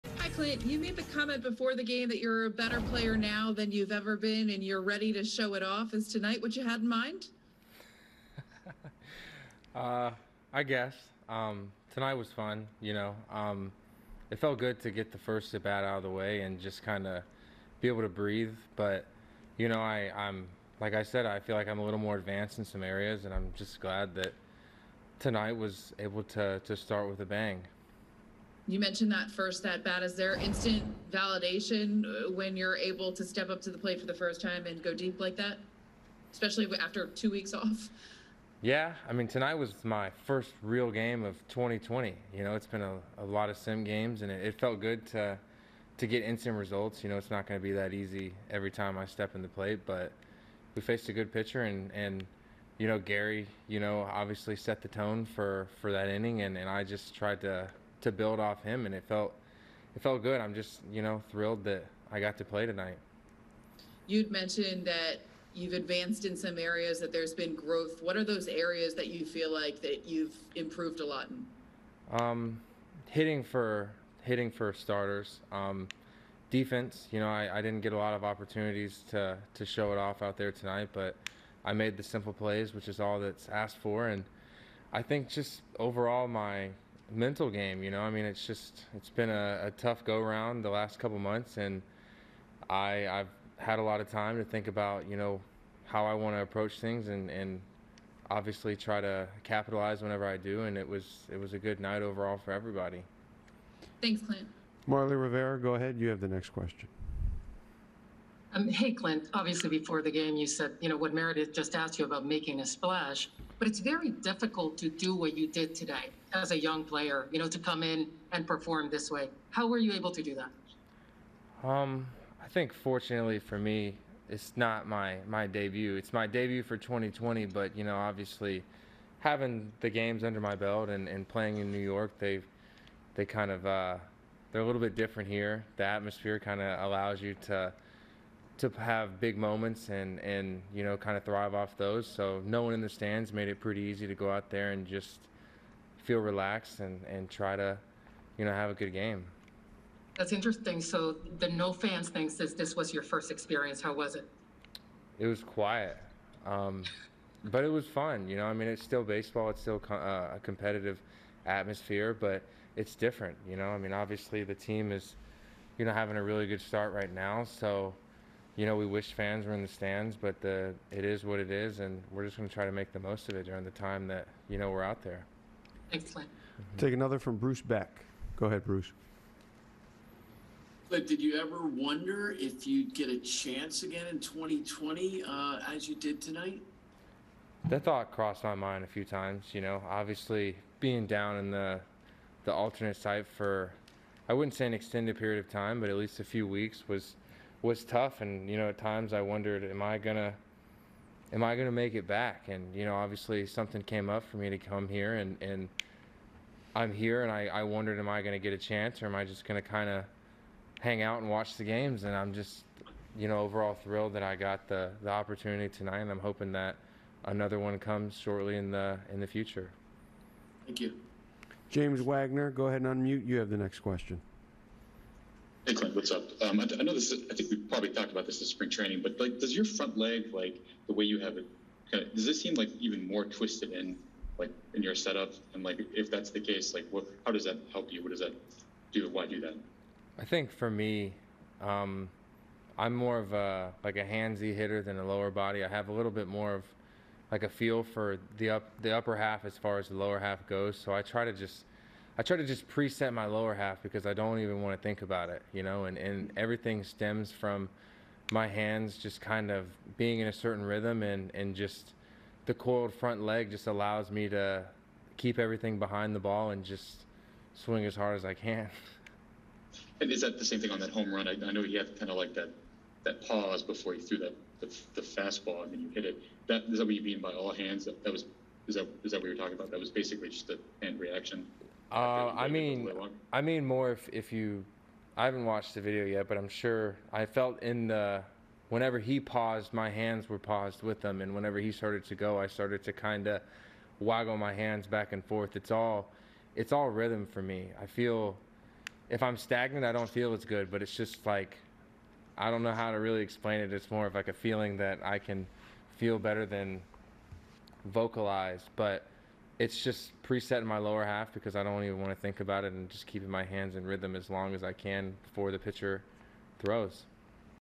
Clint Frazier goes 3-4 with a HR in season debut
Clint Frazier marked his return to the Yankees lineup with a bang, going 3-for-4 with a home run and a double en route to a 6-3 Yankees victory.